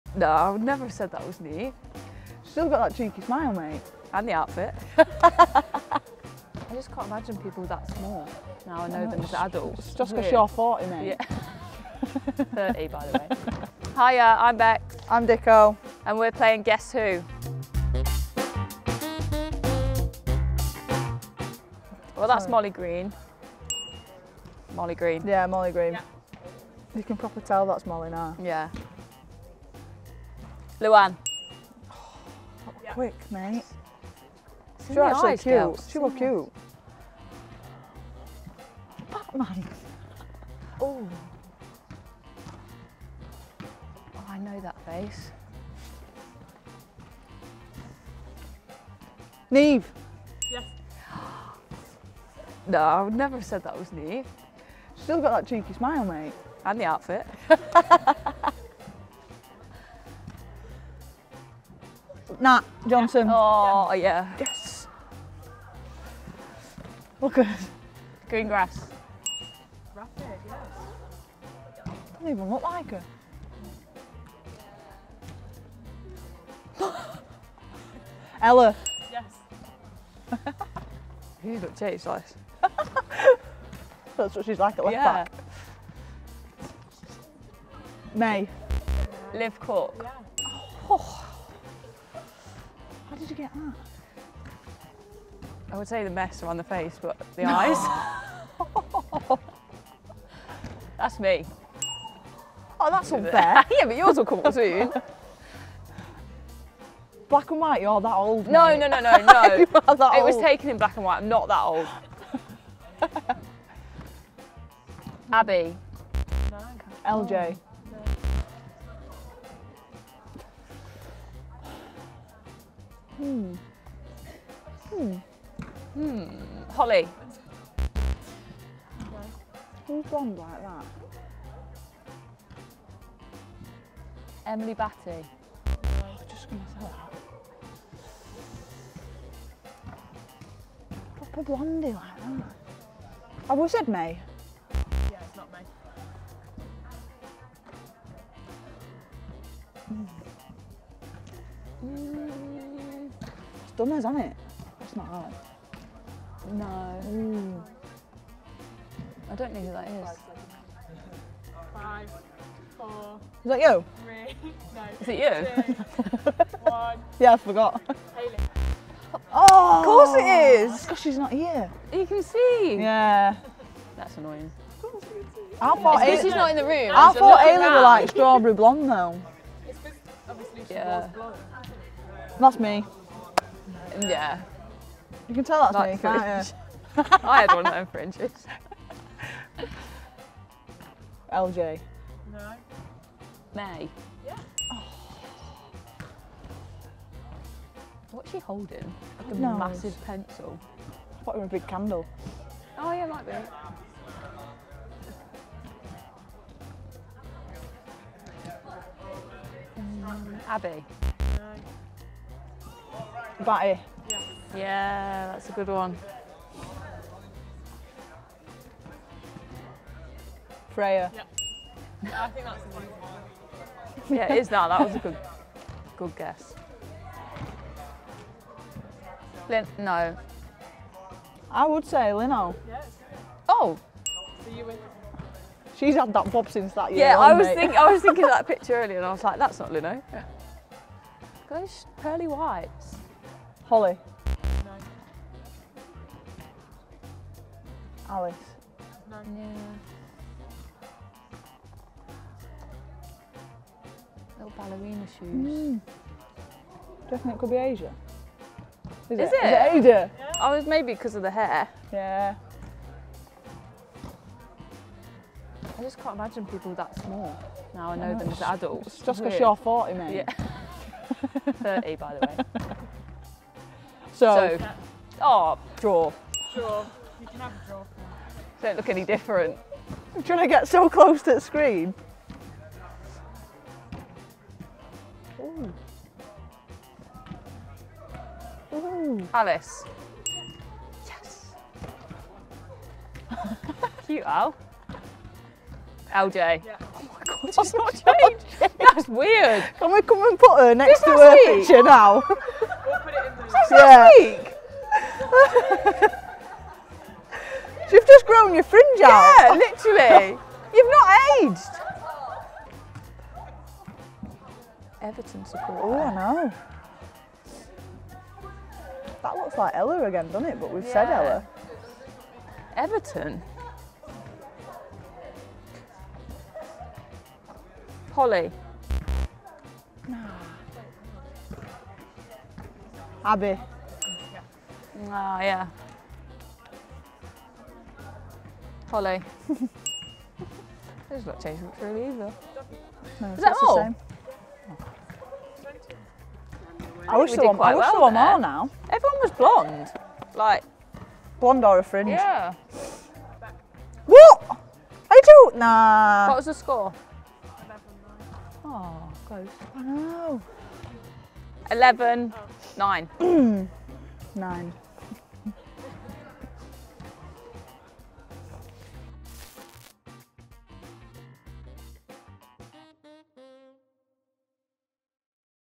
GUESS WHO | BECKY ANDERSON & CHLOE DIXON
Nottingham Forest Women players, Becky Anderson & Chloe Dixon, try to identify their teammates from their childhood pictures in 'Guess Who'.